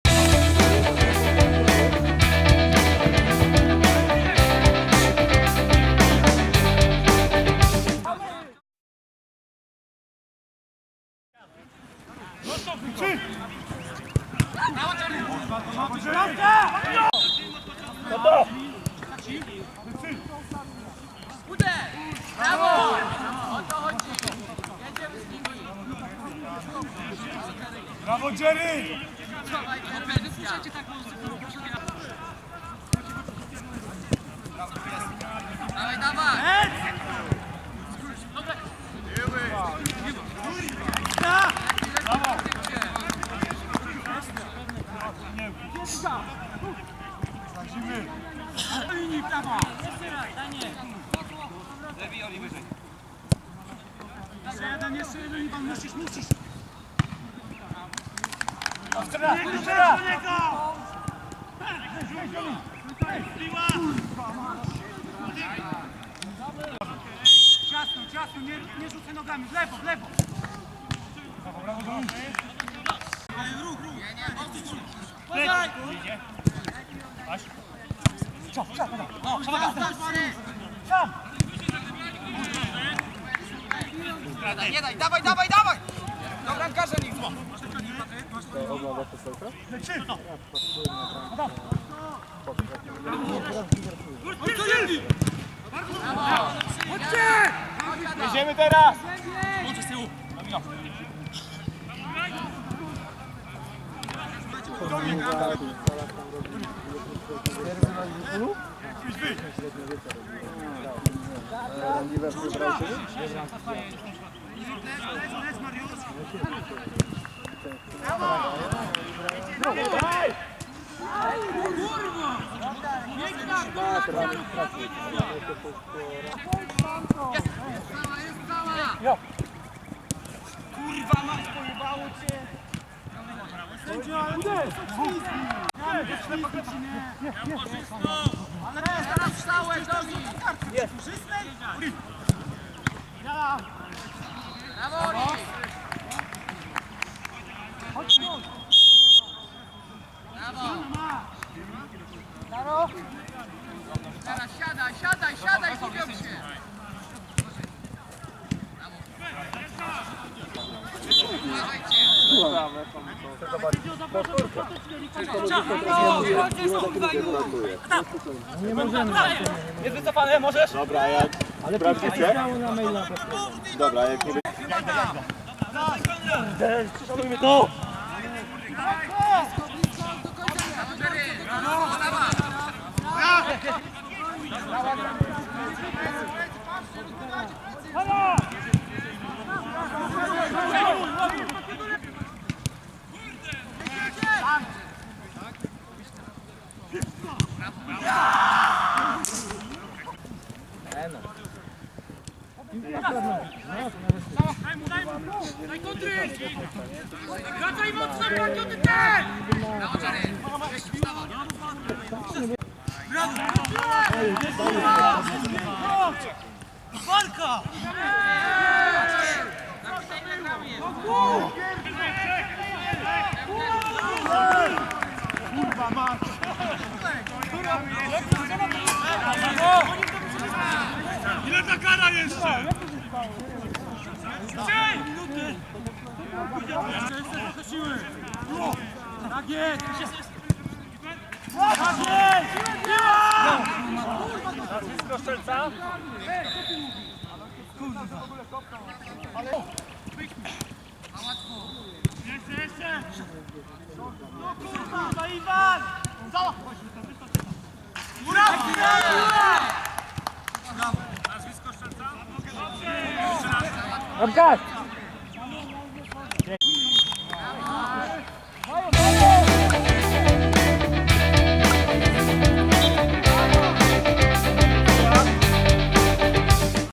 Browary Hills 32040 - Virtus Logistics: 7. tydzień (FLS Wiosna 2018)
Skrót meczu: Browary Hills 32040 - Virtus Logistics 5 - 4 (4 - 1)